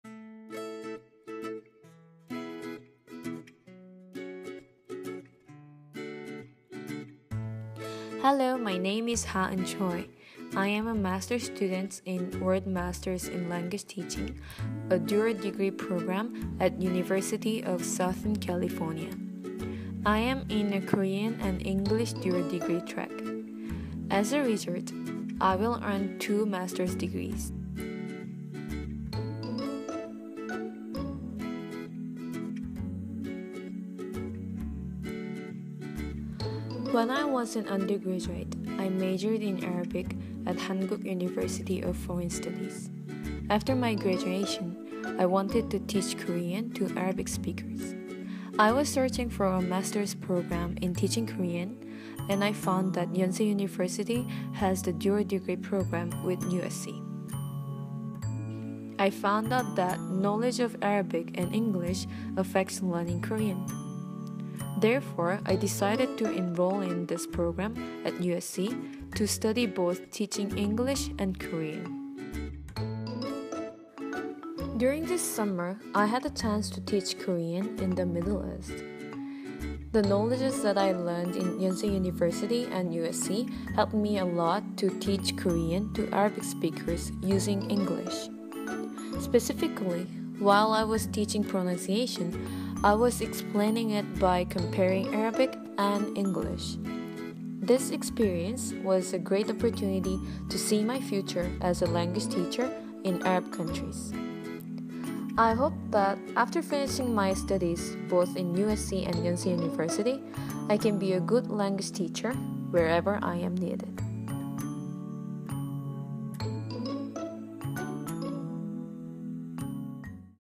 WMLT Student Feature: Ha Eun Choi
Ha Eun Choi is a World Masters in Language Teaching program student who will be getting her dual masters degrees from USC and Yonsei University in Korea. She is currently teaching an intensive Korean summer course in Muscat, Oman.